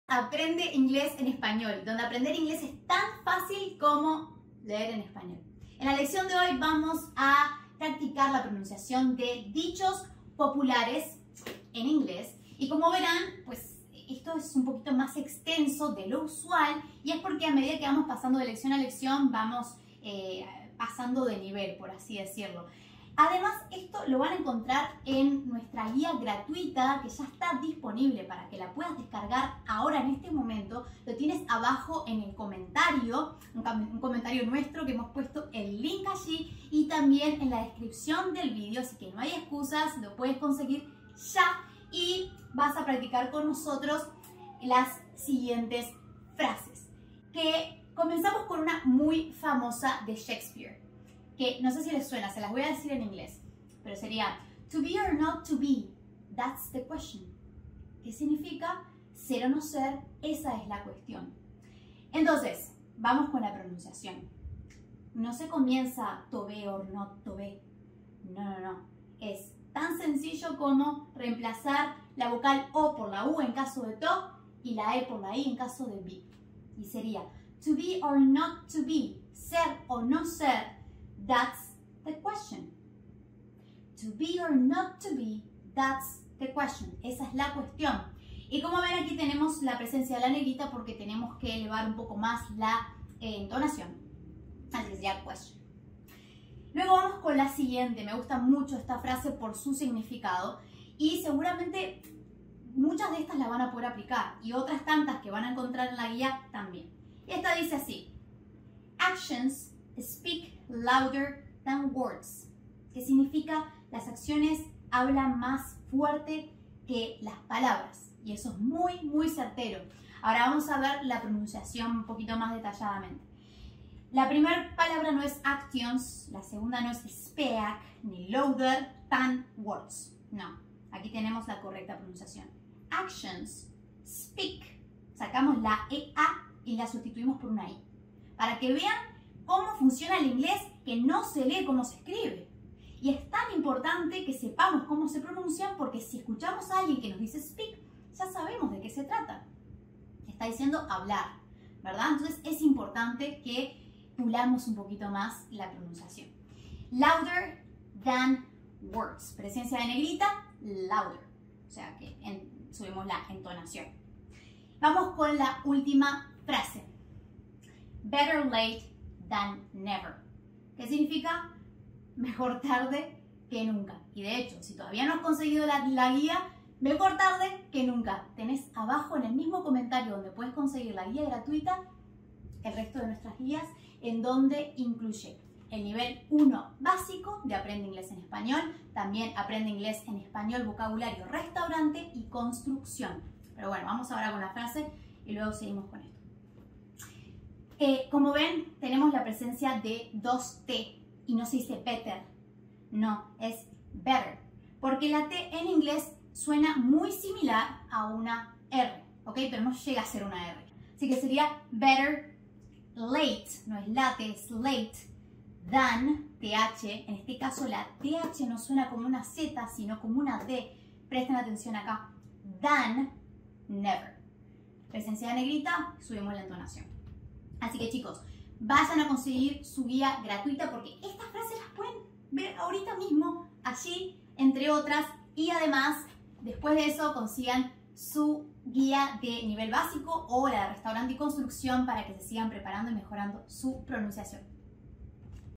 Pronunciando DICHOS POPULARES En Ingles -Aprende Ingles En Español inglesfacil │KNingles
Aprende a pronunciar: To be or not to be, that is the question - Actions speak louder than words - Better late than never   - con sonidos en Español / castellano.
👇 APRENDE INGLES EN ESPAÑOL (GUÍAS DE ESTUDIO):  👇

Contenido del video:
00:47 Cómo pronunciar: TO BE OR NOT BE, THAT IS THE QUESTION
01:36 Cómo pronunciar:  ACTIONS SPEAK LOUDER THAN WORDS
02:43 Cómo pronunciar:  BETTER LATE THAN NEVER

Frases a estudiar:
To be or not to be, that is the question